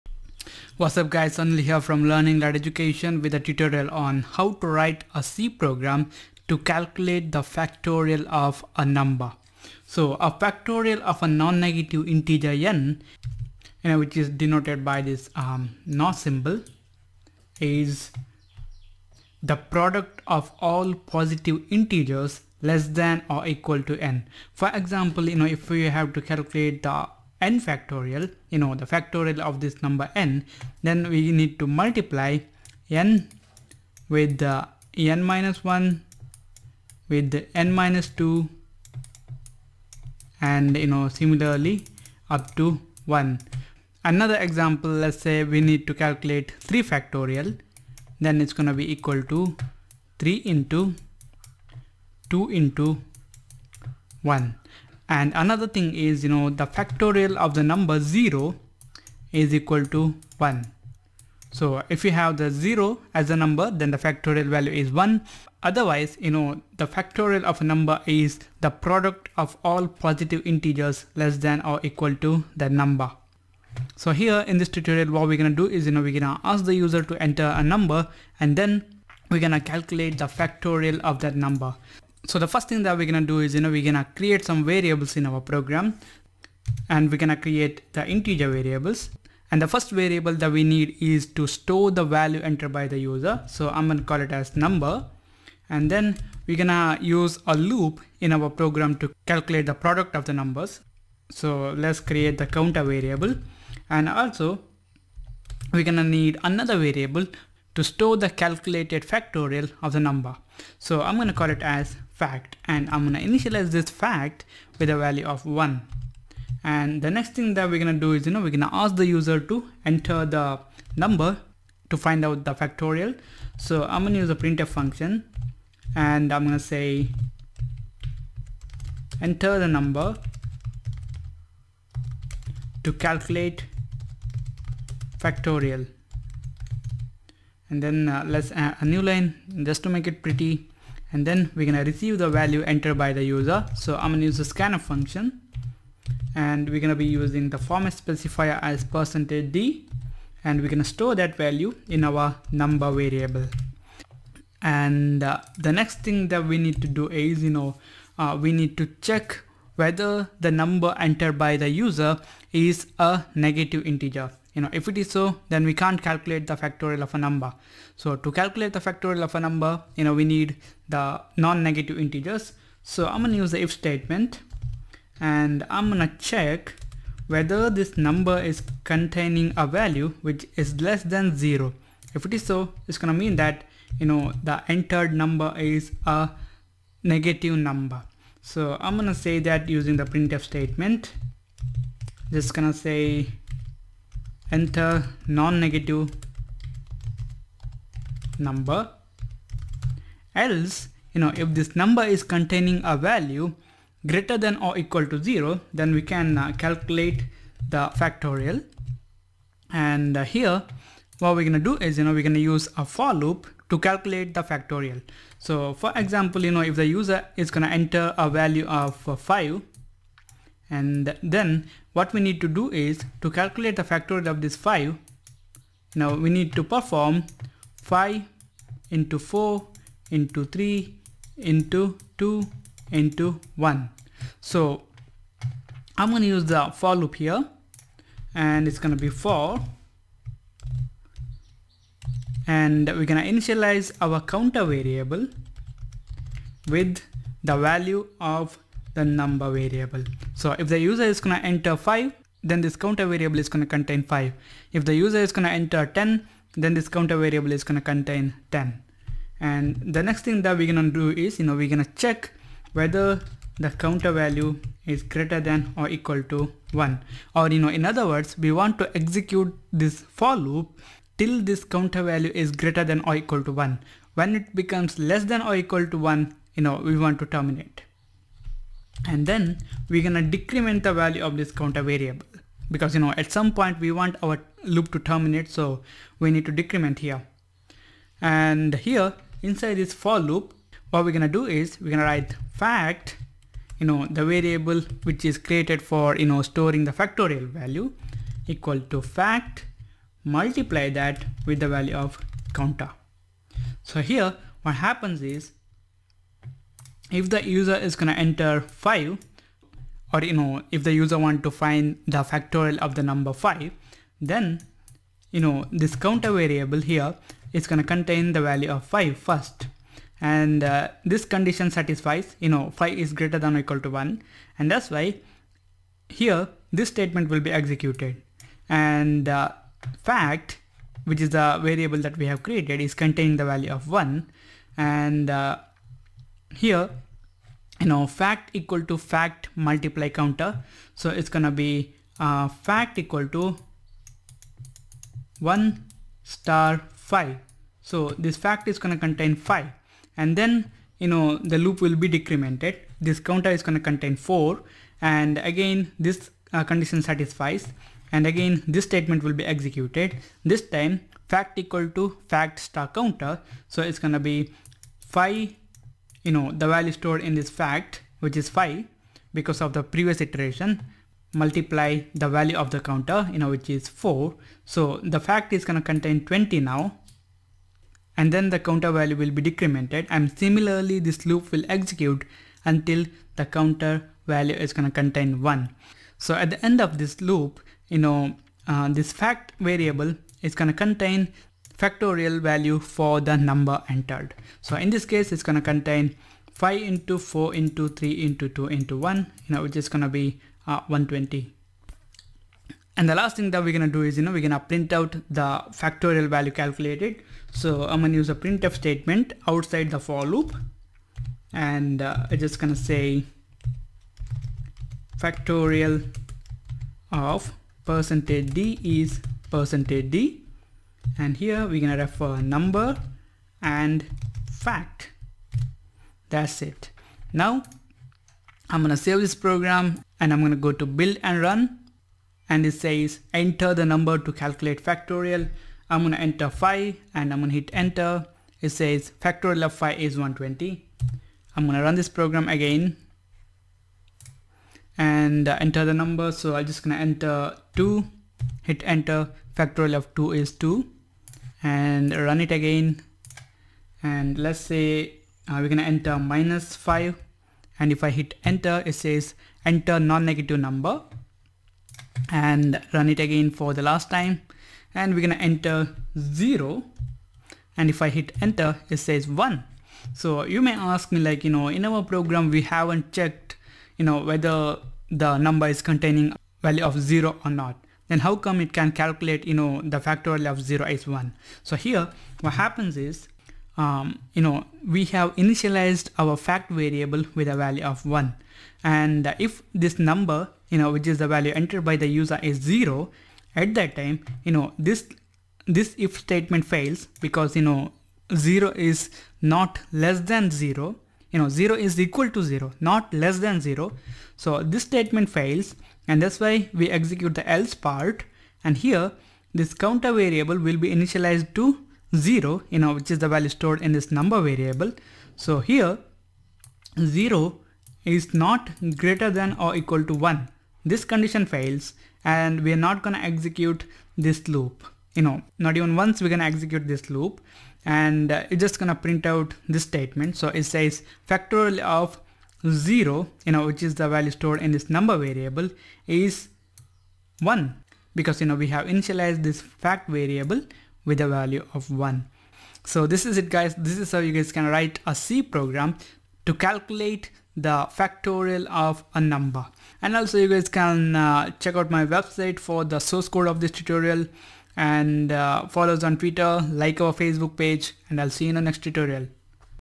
C Programming Exercise - Factorial Program Video Tutorials
In this c / cpp programming language video tutorial / lecture for beginners, you will learn how to write a program to find the factorial of a number entered by the user.

In this tutorial we are gonna ask the user to enter the number to find factorial. Then we calculate the factorial for the entered number and display it back to the user.